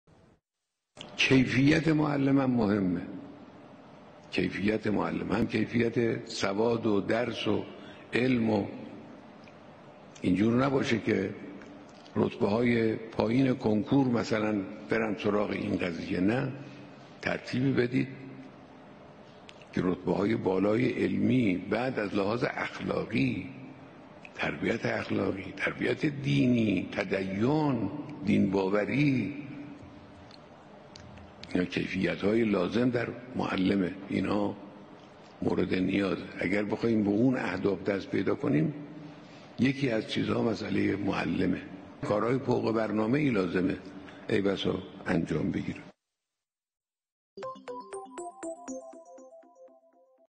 رهبر ایران: ترتیبی بدهید که رتبه‌های بالای کنکور بیایند دنبال معلمی
🔻ما از لحاظ کمیّت معلمان دچار مشکلیم.  گزارشی که به من دادند این است که الان حدود هفتاد هزار معلم ما در شرف بازنشستگی‌اند، در حالی‌که حدود نصف این جمعیت را می‌توانیم جذب کنیم. مشکل بزرگی است. باید روی این قضیه کار کرد. کیفیت معلم هم مهم است. کیفیت سواد و درس و علم و...  این جور نباشد که رتبه‌های پایین کنکور بروند سراغ این قضیه. ترتیبی بدهید که رتبه‌های بالای علمی بیایند و از لحاظ اخلاقی، تربیت اخلاقی، تربیت دینی، تدین، دین‌باوری قوی باشند